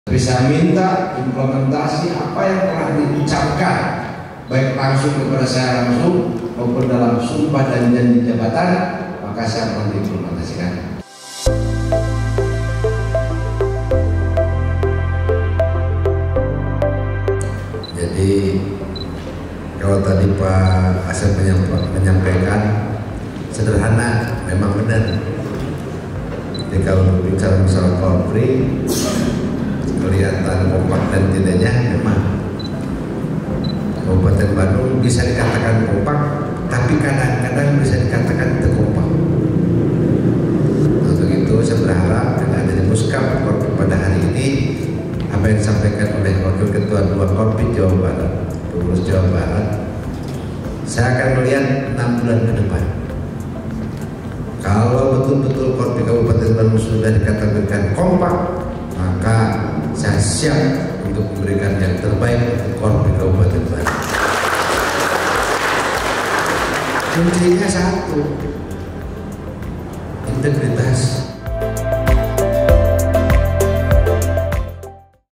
Pembukaan Muskab VII KORPI Kab. Bandung tahun 2023
Bupati menyampaikan pesan kepada pengurus KORPRI untuk meningkatkan pelayanan terhadap masyarakat karena akan dihadapkan dengan ekspetasi dari masyarakat yang lebih besar, dan meminta untuk lebih cepat, inovatif, dan kreatif dalam menyelesaikan persoalan.

Tak lupa bupati juga menyampaikan terima kasih dan penghargaan kepada pengurus lama, dan ucapan selamat kepada pengurus baru semoga bisa bekerja, bersinergi dan berinovasi membawa KORPRI menjadi organisasi ASN yang bisa menaungi, mendorong dan mengajak ASN secara bersama-sama  mewujudkan visi pemerintah daerah, yaitu terwujudnya Kabupaten Bandung yang BEDAS (bangkit, edukatif, dinamis, agamis dan sejahtera).